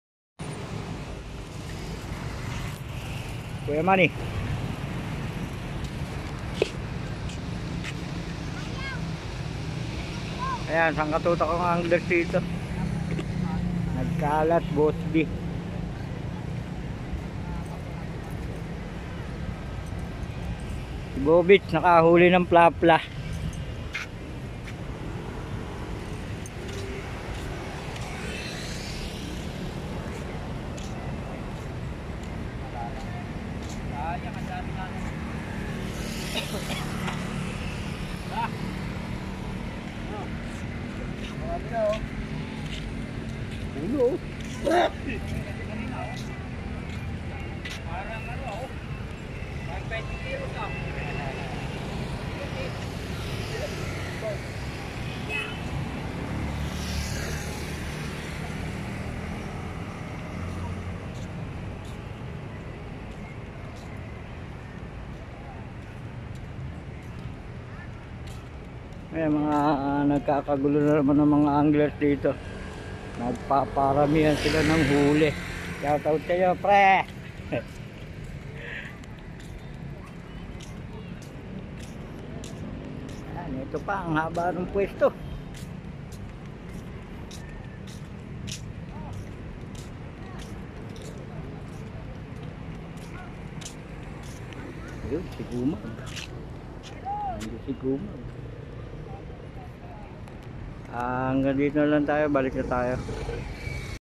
marikina tiksay dante's vlog
mga angler's humuhuli sila ng tilapia marikina river.